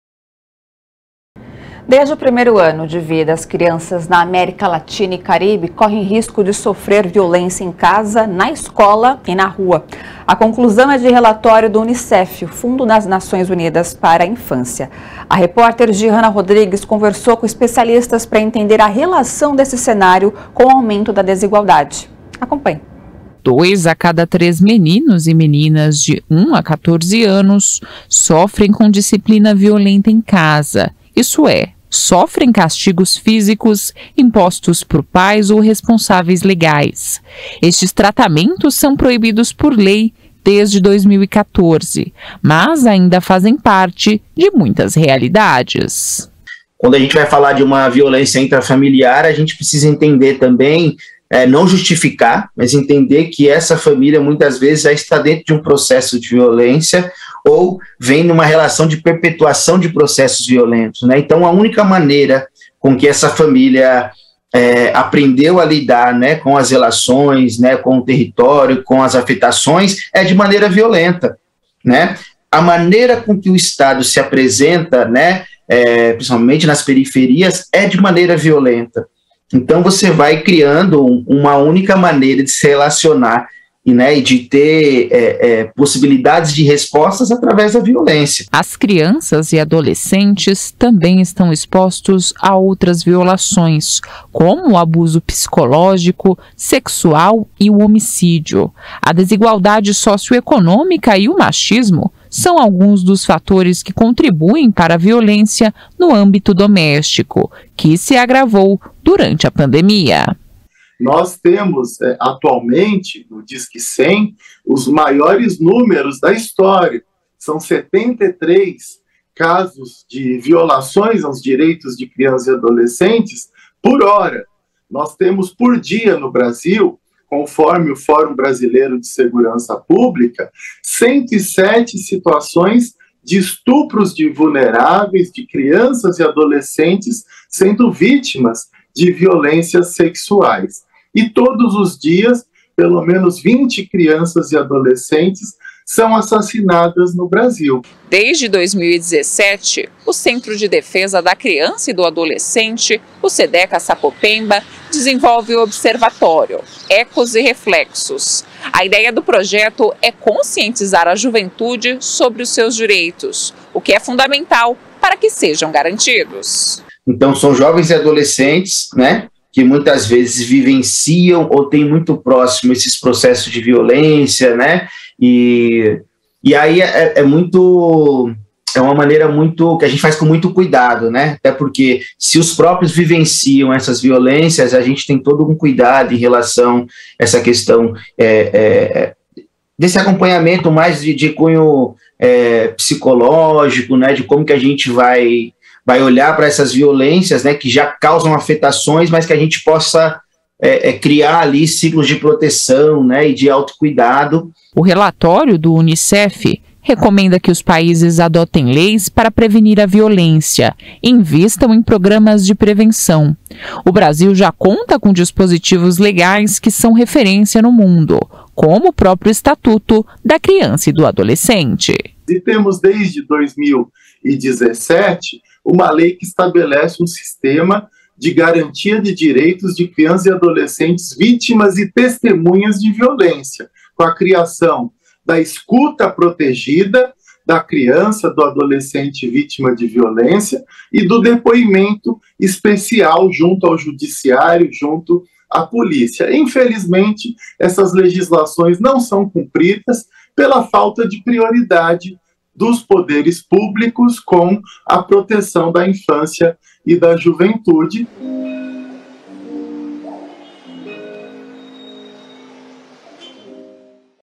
Crianças latinas e caribenhas estão mais expostas à violência
Desde o primeiro ano de vida, as crianças na América Latina e no Caribe correm risco de sofrer violência em casa, na escola e na rua. A conclusão é de relatório do Unicef, o Fundo das Nações Unidas para a infância.